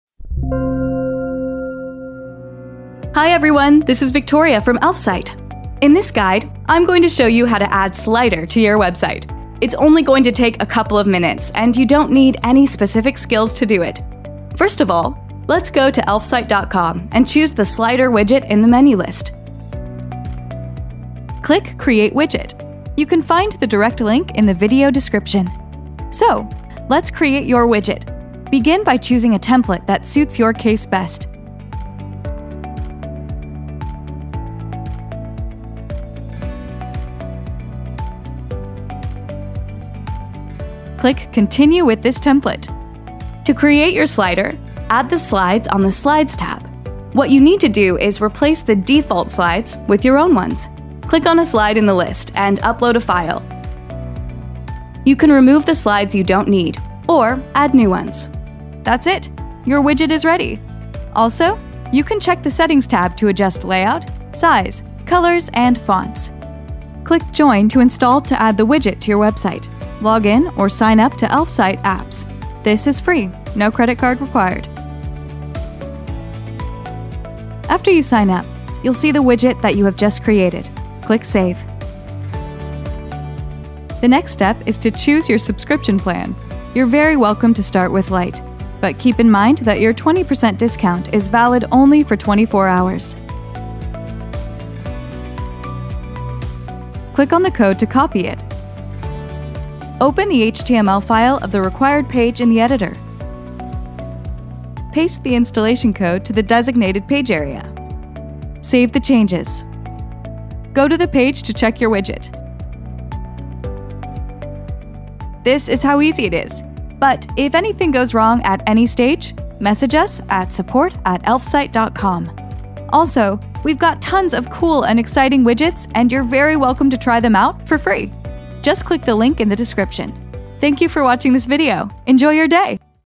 How to Add Slider to HTML
Add a Slider widget to HTML site in about a couple of minutes -
No coding, no sweat. Free and easy.

Advantages:
• With the help of an astonishing special effect, make photos and videos indispensable for visitors.
• Your designs and marked works are highlighted by means of a slider.
• All the most splendid content is in one certain place to make exploring your website easier.

How to embed a Slider to HTML website?
In order to insert the slider on your website, take three quick actions:
1) Create and adjust the widget;
2) Save the personal installation code which you will see in the popup;
3) Paste this code on your website.